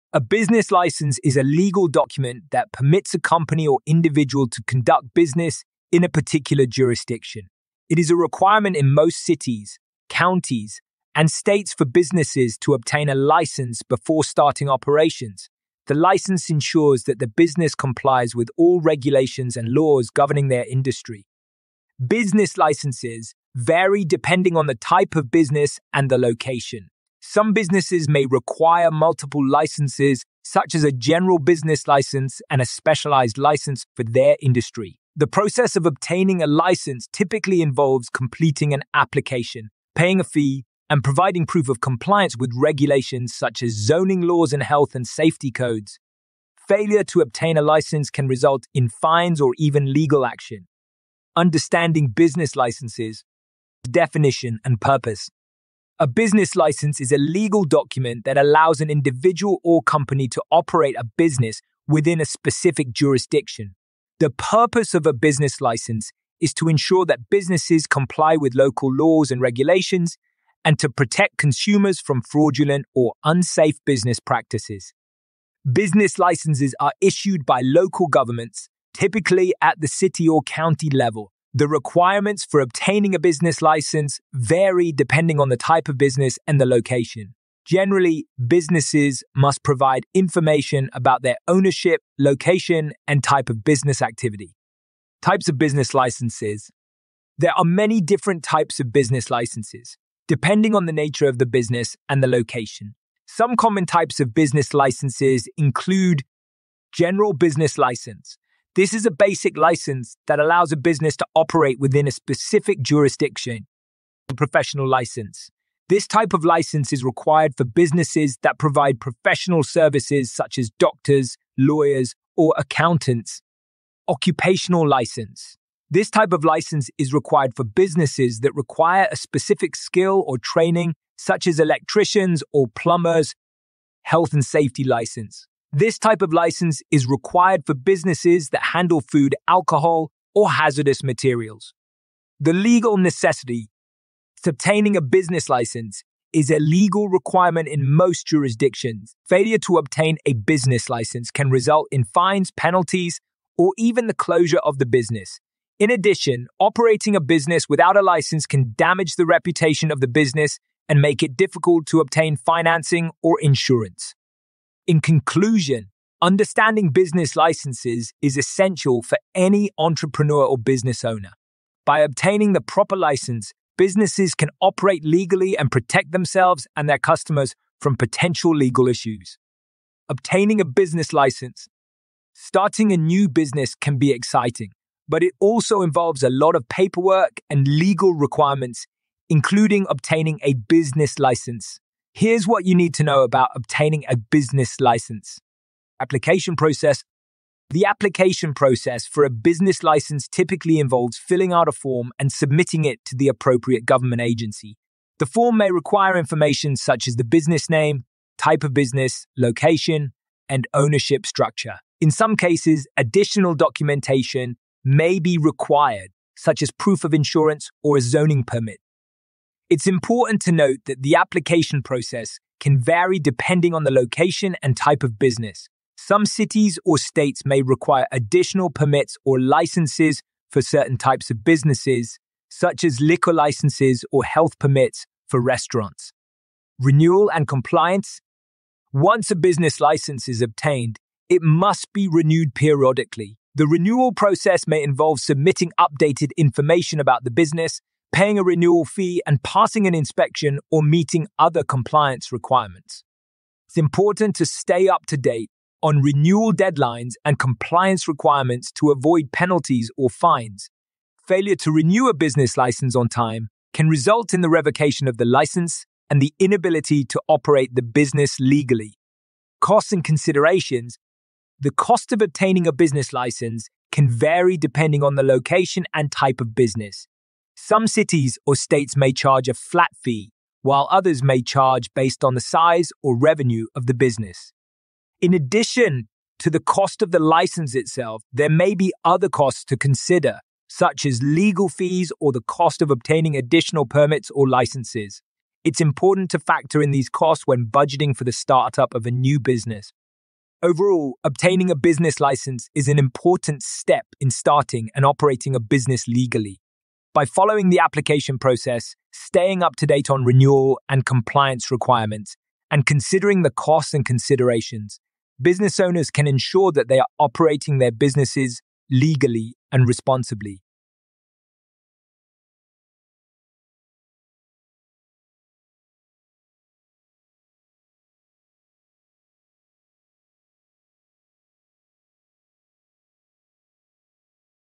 What is a Business License: A Clear Explanation
A business license is a legal document that permits a company or individual to conduct business in a particular jurisdiction. It is a requirement in most cities, counties, and states for businesses to obtain a license before starting operations. The license ensures that the business complies with all regulations and laws governing their industry.